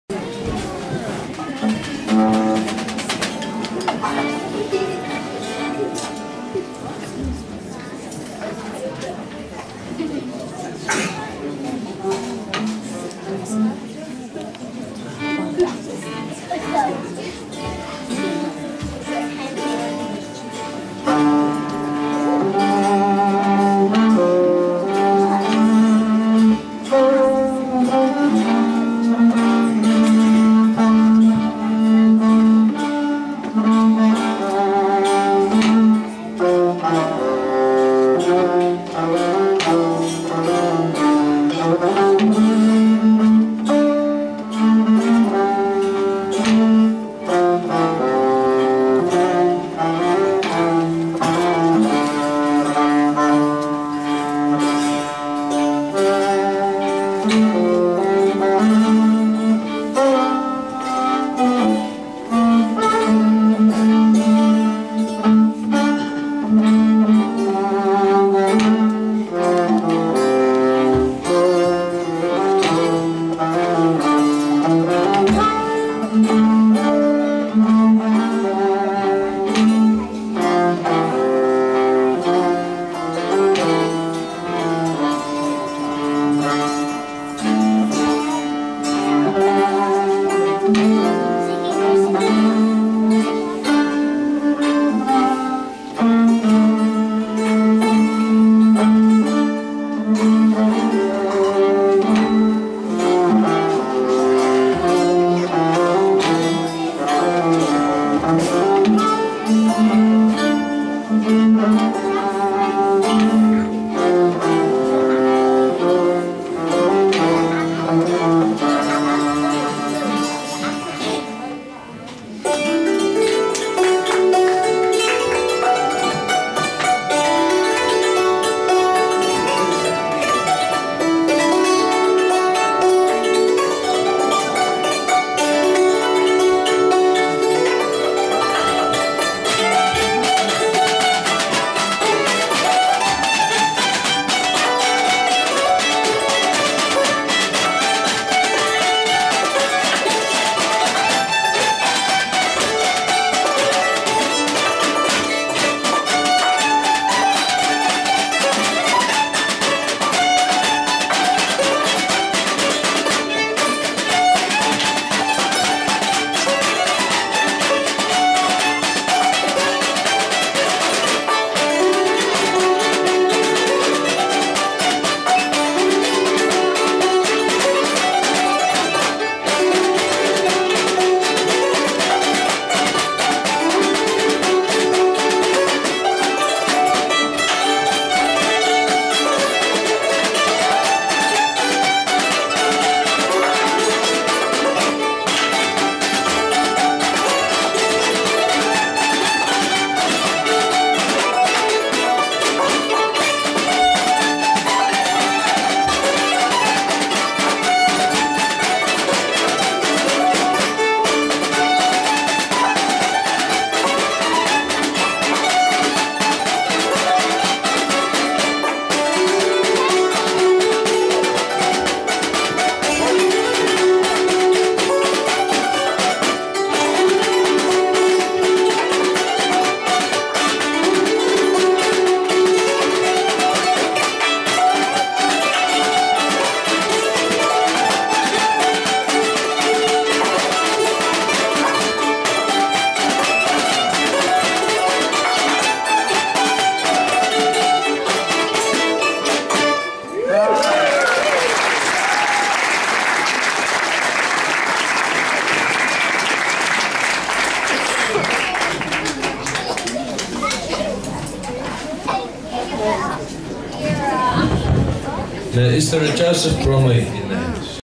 2012 Roses Gap Music Camp - Tutor's Concert - John Madin
What an instrument and what a sound !  Andy Rigby on the Hammer Dulcimar is a firm favourite.